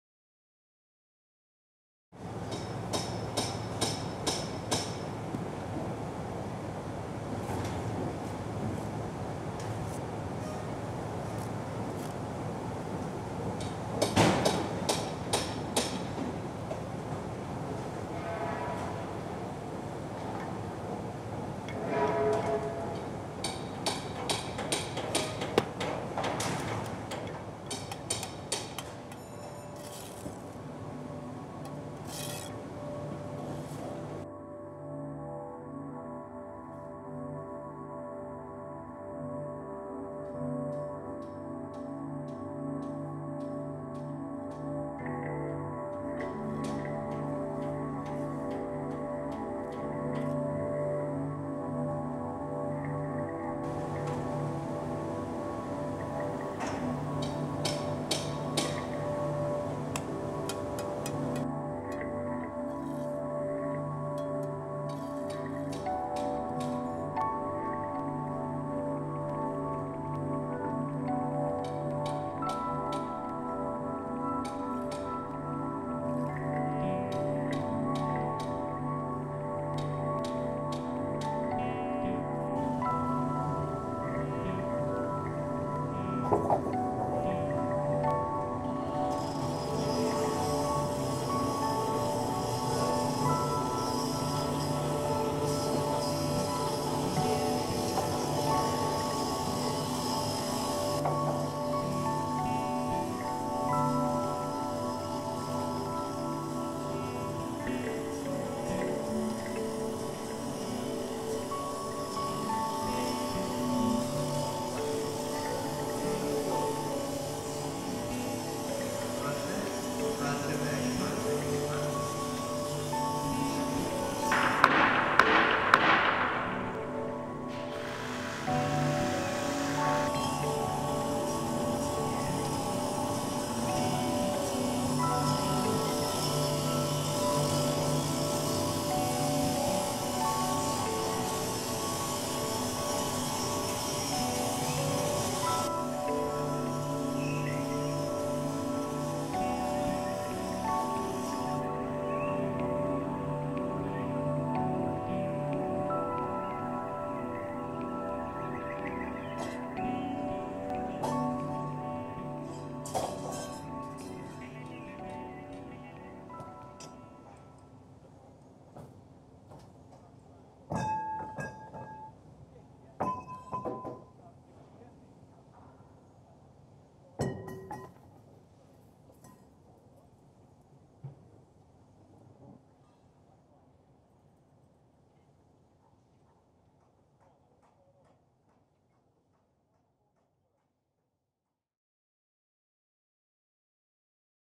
dietrich untertrifaller architekten | tum campus im olympiapark
construction site film about the new building of the TUM campus by dietrich | untertrifaller architekten. the building is located in the olympiapark in munich. characteristic for the architecture of dietrich | untertrifaller architekten is the use of wood. the TUM is particularly impressive because of the enormous wooden roof.

9sekunden is a team of young cineasts, who have combined their love for films with their love for good architecture and nature.